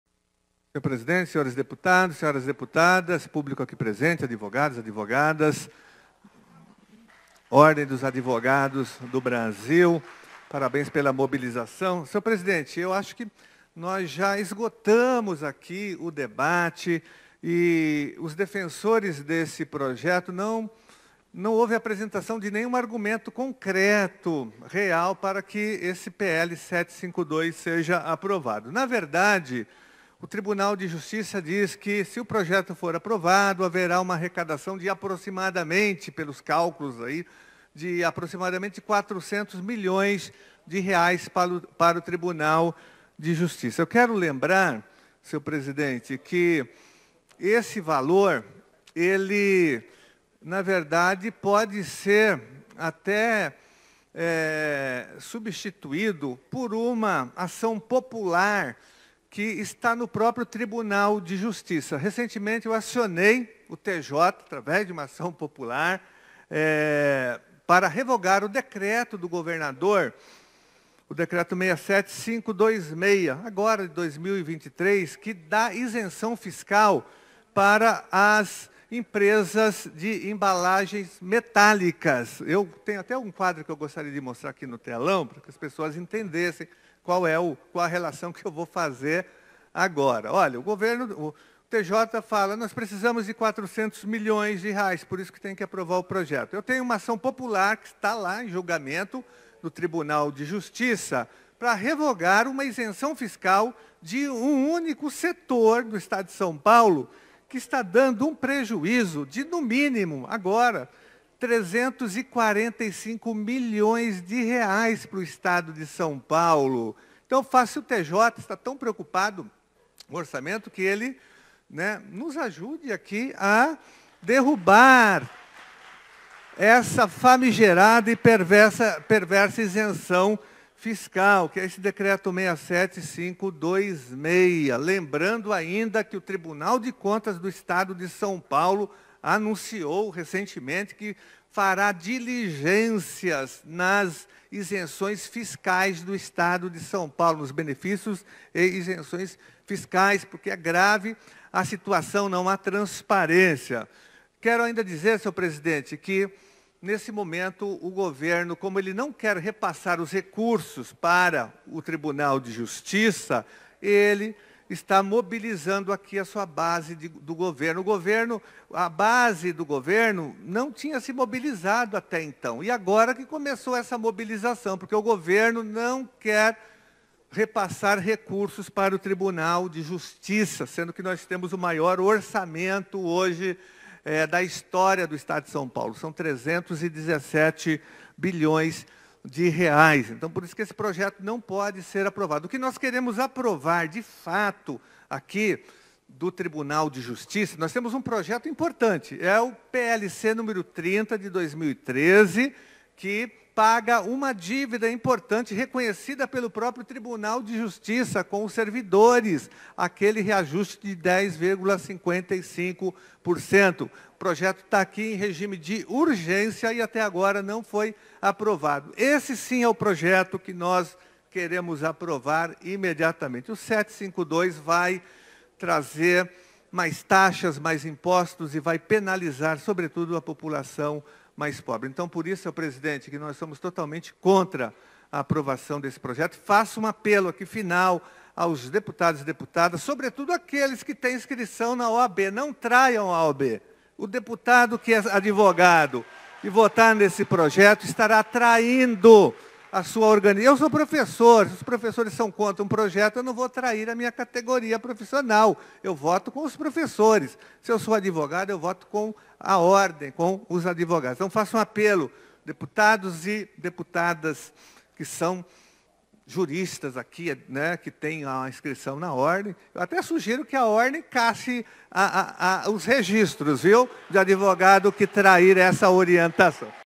Contra o PL 752 que aumenta as custas judiciais
Giannazi criticou a falta de argumentos para a aprovação do projeto de lei 752, destacando que o Tribunal de Justiça estima uma arrecadação de cerca de 400 milhões de reais caso o projeto seja aprovado. O deputado ressaltou sua ação popular para revogar um decreto de isenção fiscal que causa prejuízo ao estado de São Paulo e pediu aos deputados que não traiam a Ordem dos Advogados do Brasil ao votar a favor do projeto.